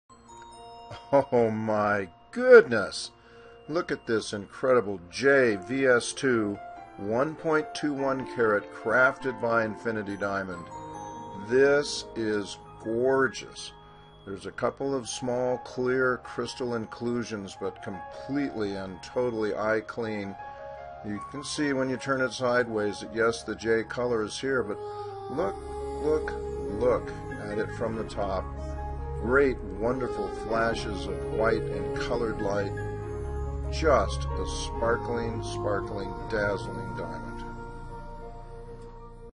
1.21ct J VS2
It is a amazing how wonderful a J color can look when the diamond has been cut with utmost optical precision the way these Crafted by Infinity diamonds are.